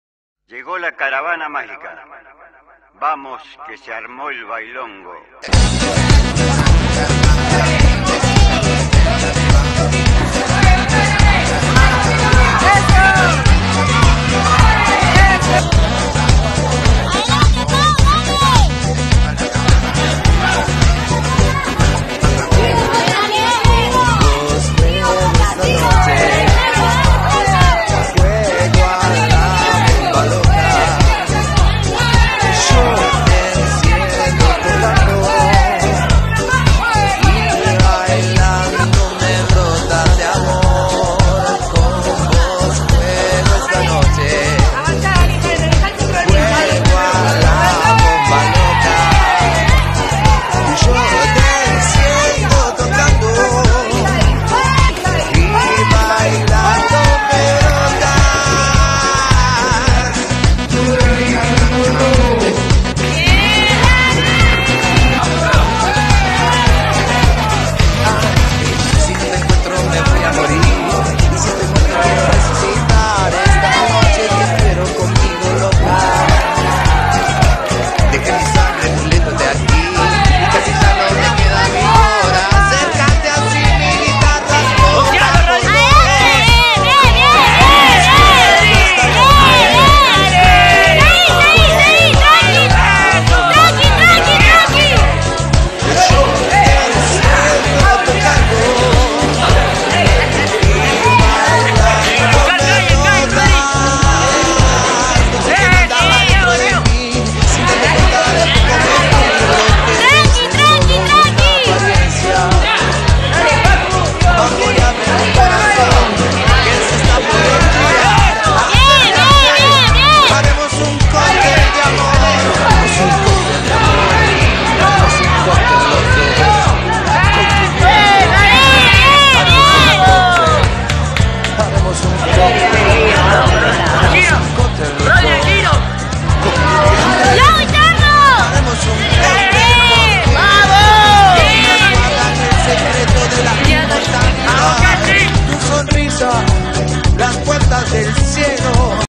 THAI23-GYM
Torneo realizado en moron el 9 de febrero 2020 donde el equipo dijo presente:

Somos THAI23-GYM.